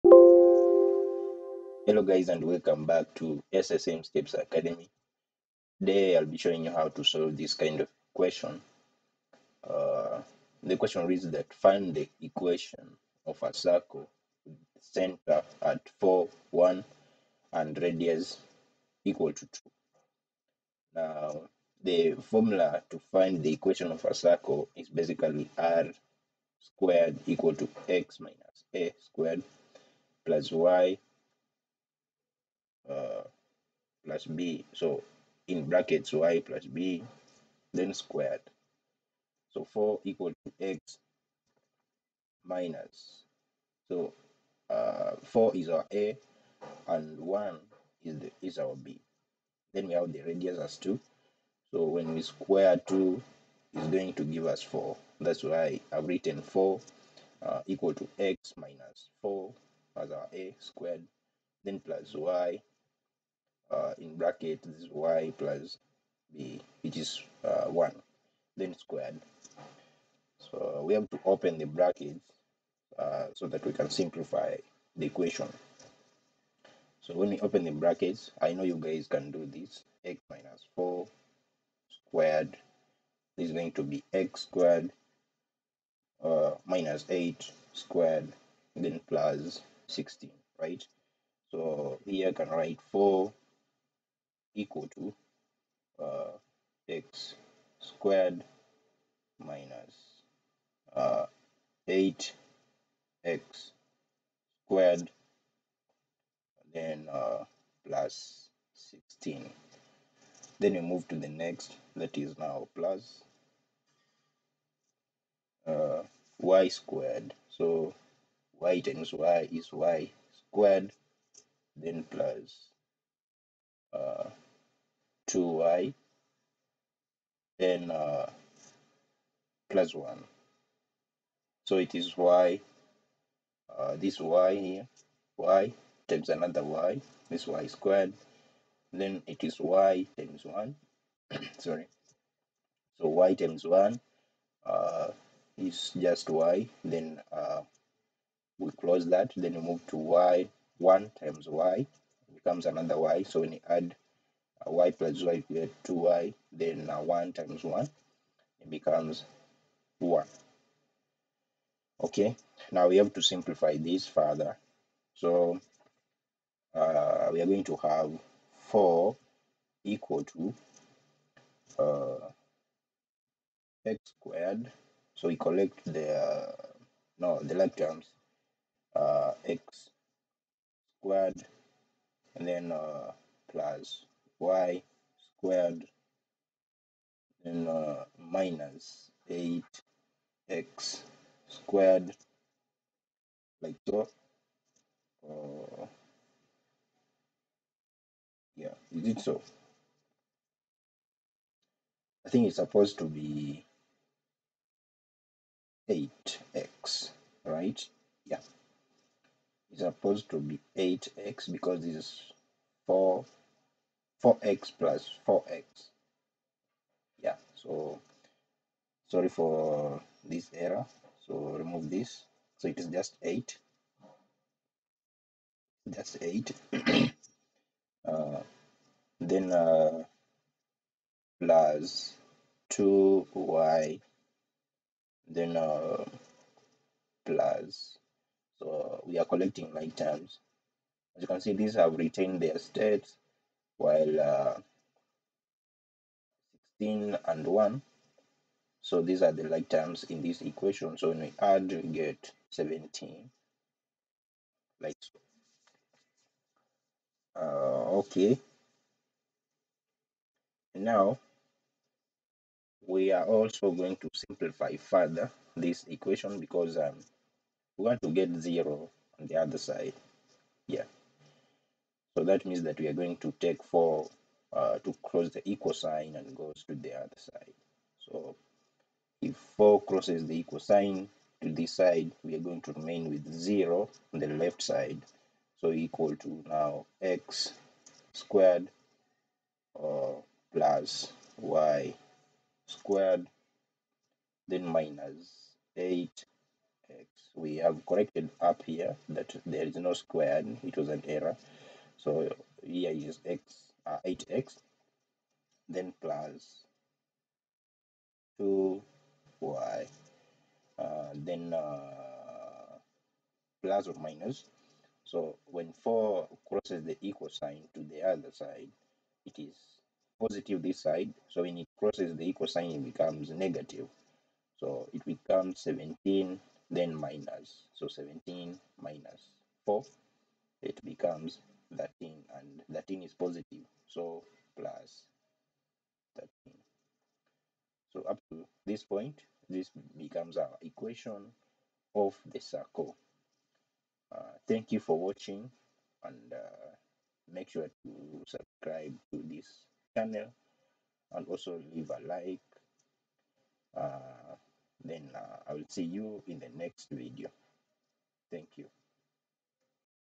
Find The Equation of a Circle.
This video teaches you how to find the equation of a circle when given the radius and the center.